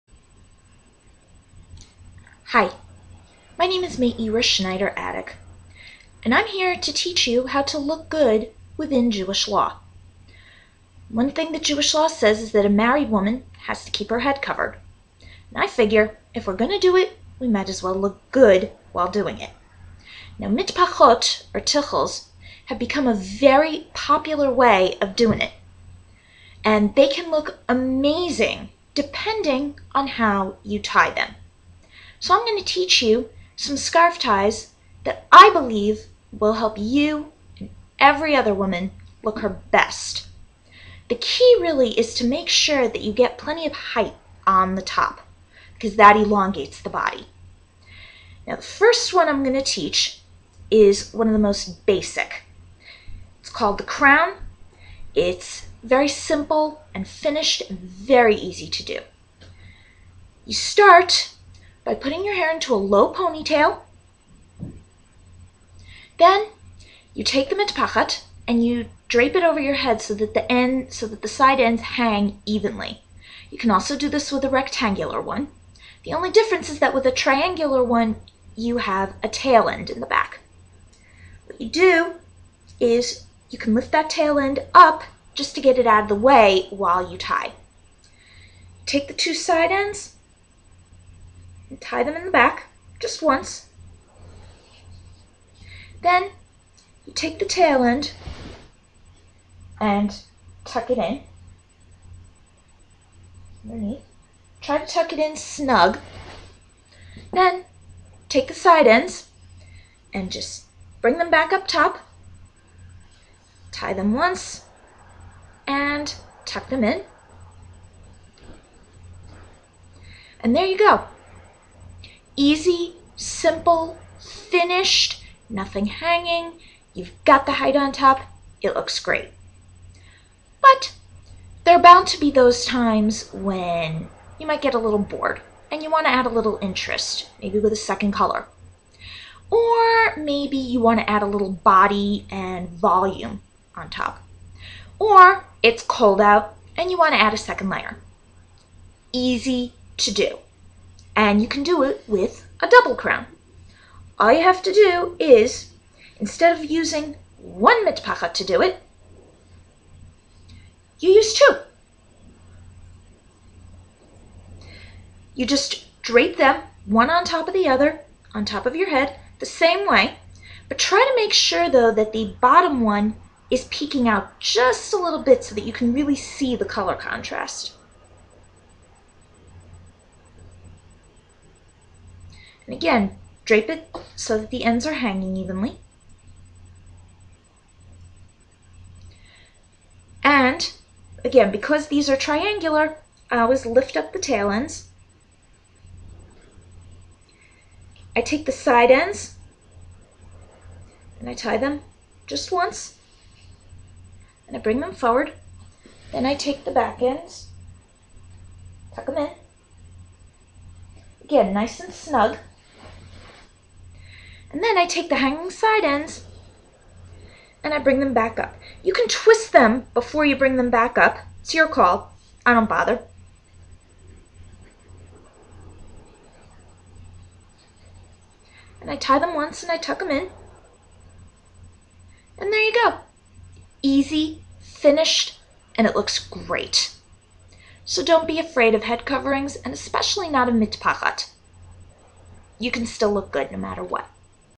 Look Your Best in Mitpachot (Tichels)- Crown tie
This is how to tie your mitpachat into a "crown" tie. Very easy and simple, and it looks great.